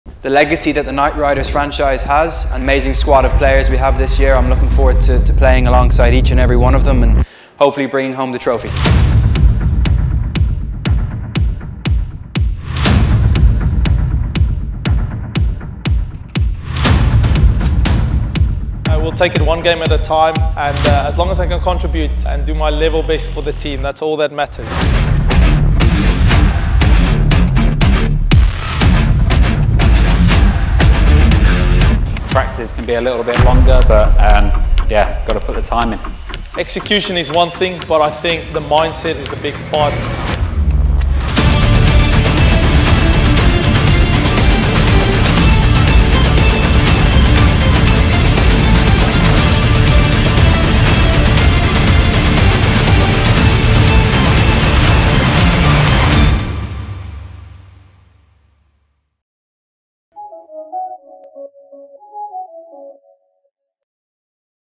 Geared Up For The Play-offs
The Abu Dhabi Knight Riders are all geared up for the play-offs as they edge closer to their quest to become the champions 🏆
Their determination knows no bounds, as they are AllInForCricket 💪🏏